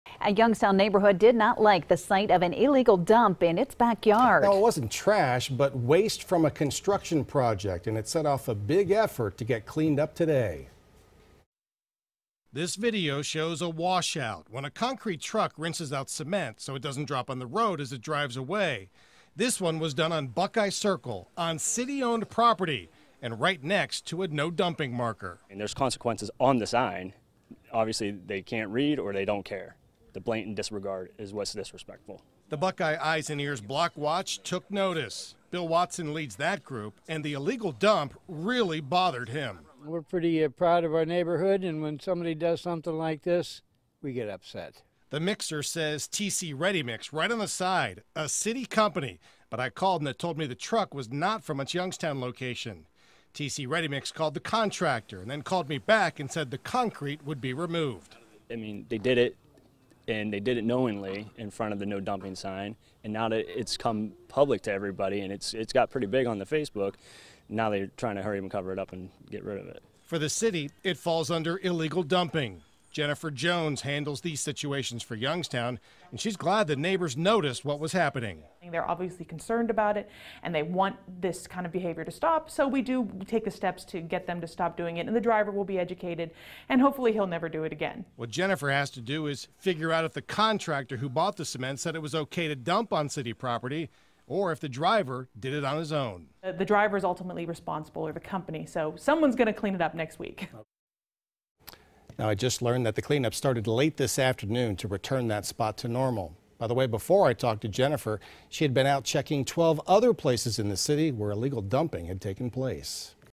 Neighborhood demands cleanup of illegal concrete dump in Youngstown
"We're pretty proud of our neighborhood and when somebody does something like this, we get upset," Bill Watson said.